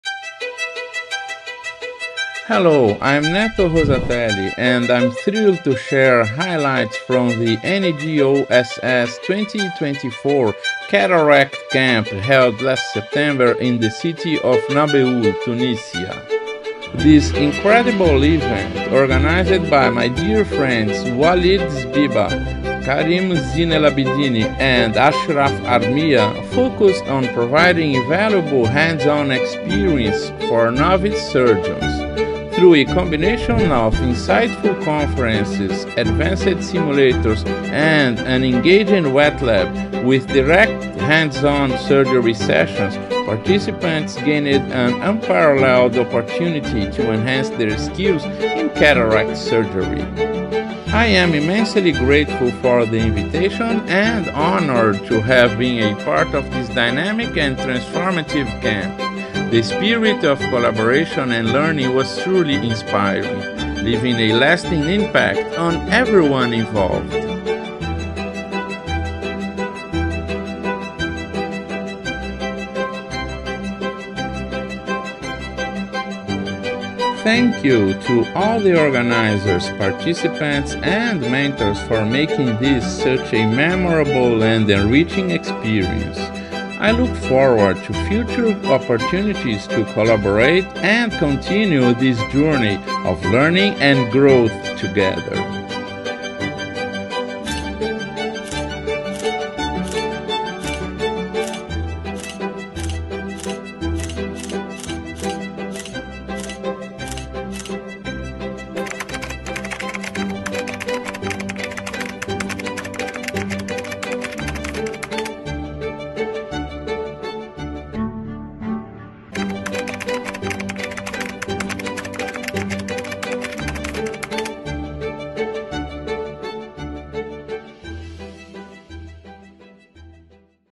Special Edition - NEGOSS 2024 Cataract Camp Tunisia
I'm thrilled to share highlights from the NEGOSS 2024 Cataract Camp held last September in the city of Nabeul, Tunisia.
This incredible event, organized by my dear friends Walid Zbiba, Karim Zinelabidine, and Ashraf Armia, focused on providing invaluable hands-on experience for novice surgeons. Through a combination of insightful conferences, advanced simulators, and an engaging wetlab, with direct hands-on surgery sessions, participants gained an unparalleled opportunity to enhance their skills in cataract surgery.
I am immensely grateful for the invitation and honored to have been a part of this dynamic and transformative camp. The spirit of collaboration and learning was truly inspiring, leaving a lasting impact on everyone involved.
Thank you to all the organizers, participants, and mentors for making this such a memorable and enriching experience. I look forward to future opportunities to collaborate and continue this journey of learning and growth together.